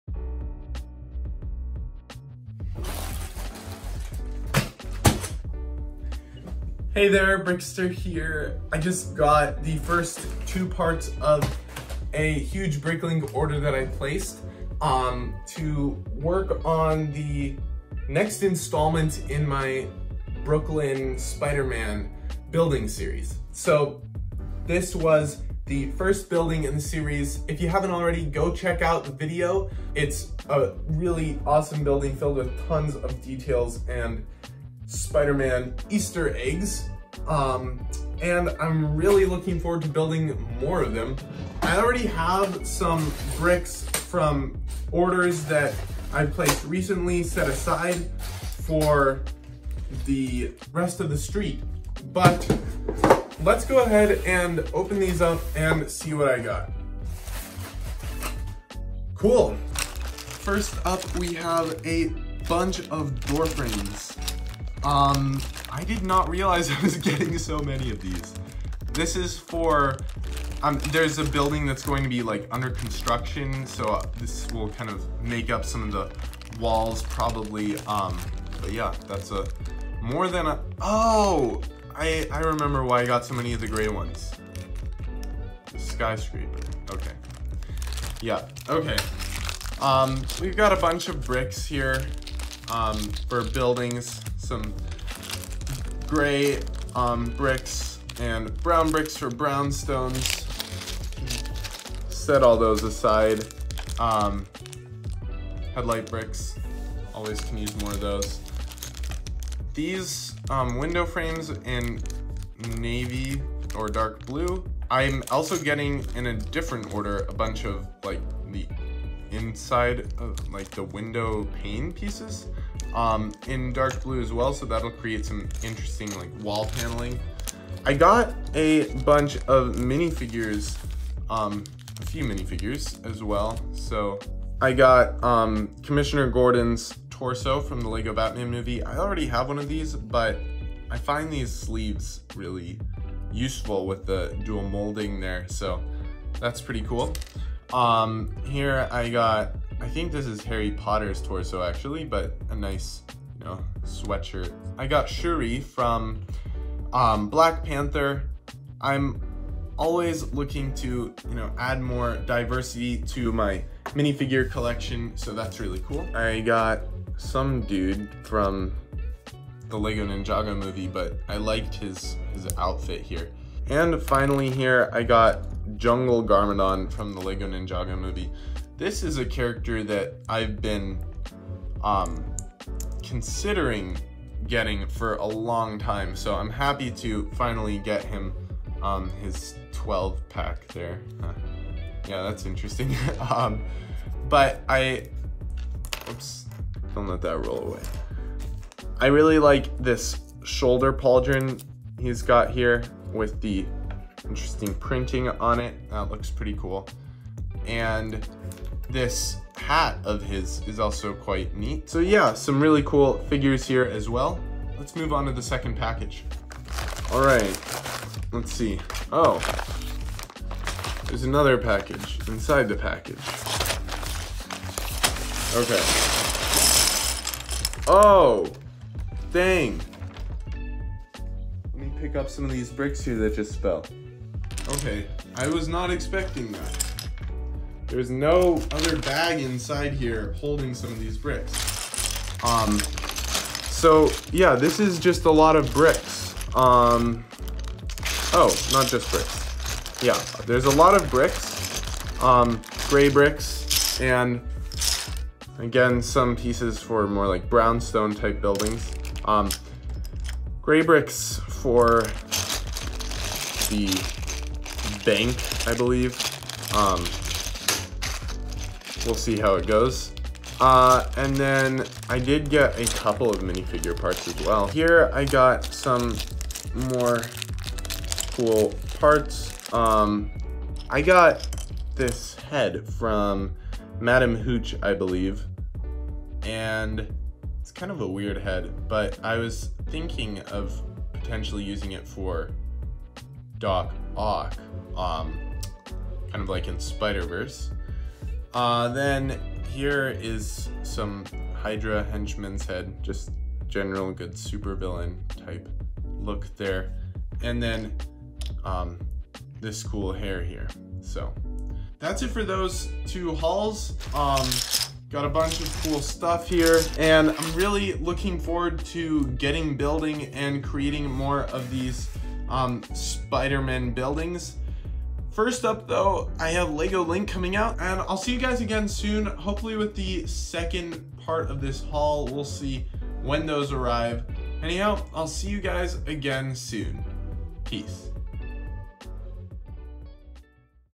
BIG Bricklink Haul Part 1
I'm so excited to get to work on the next Spider-Man buildings! Also be sure to check out LEGO Link, live on LEGO IDEAS:

Check me out on Instagram for more fun content: @brixter.productions